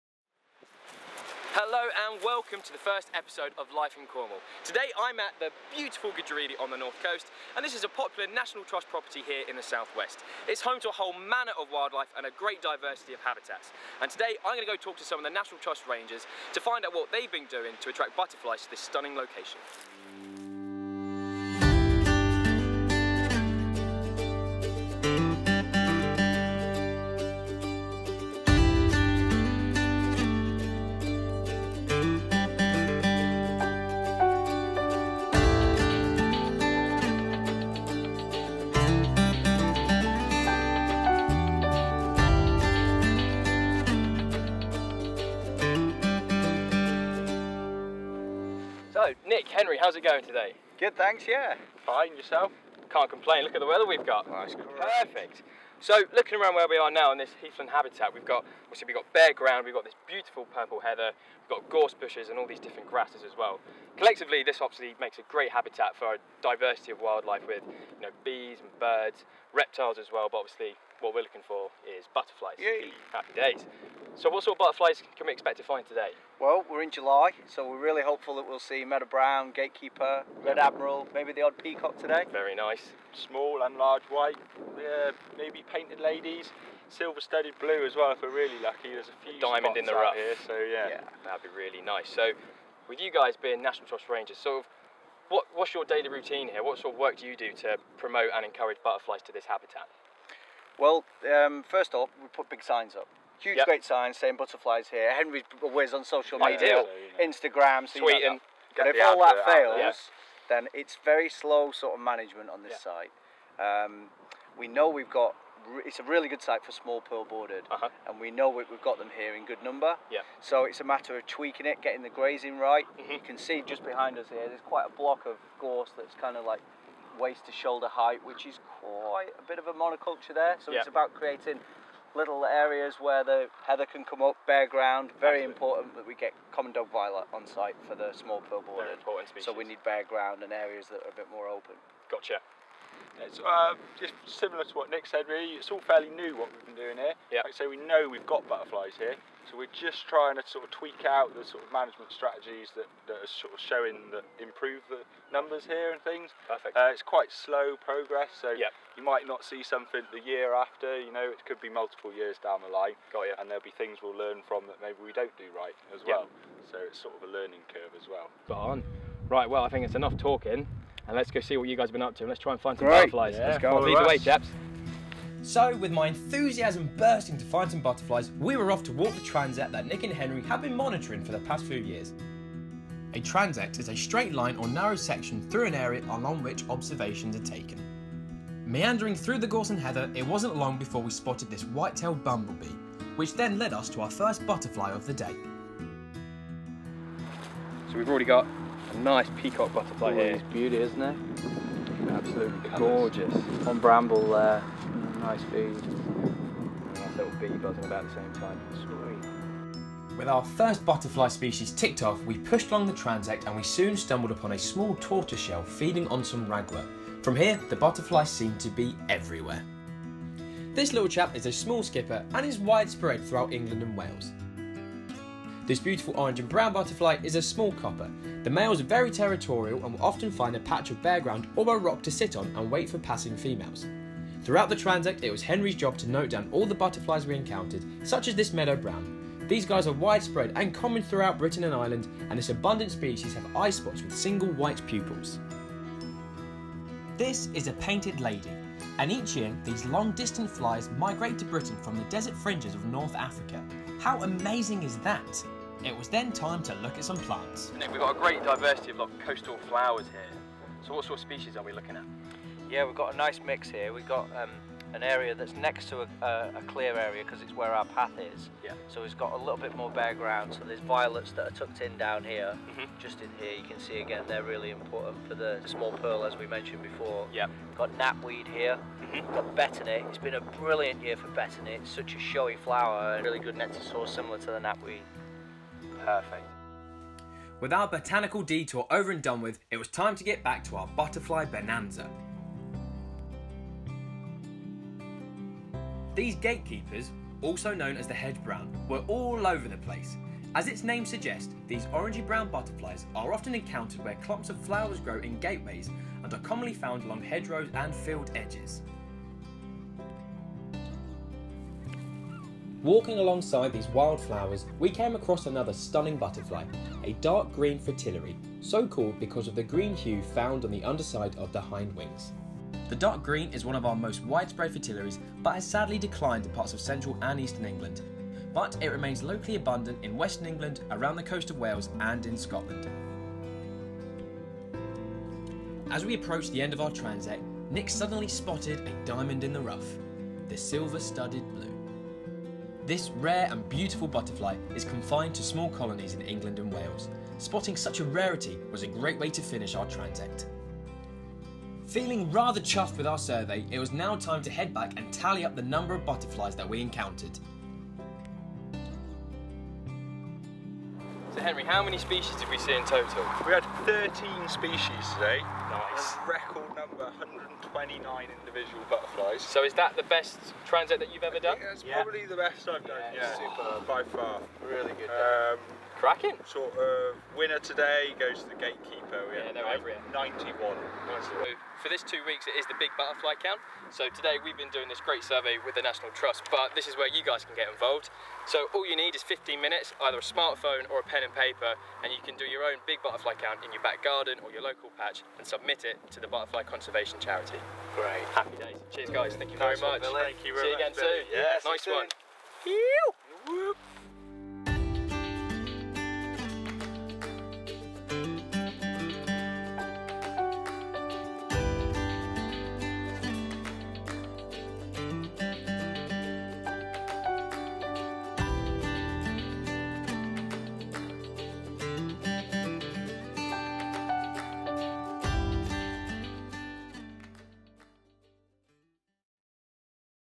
Life In Cornwall Episode 1: Butterflies
Last July the MuddyDuck crew headed out with Nick Holden and Henry Upton, from the National Trust mid-Cornwall Ranger team, to participate in one of their weekly butterfly transects as part of last years big butterfly count. So, go and grab yourself a cuppa and some biscuits and join Nick, Henry and Billy as they go on the hunt for butterflies.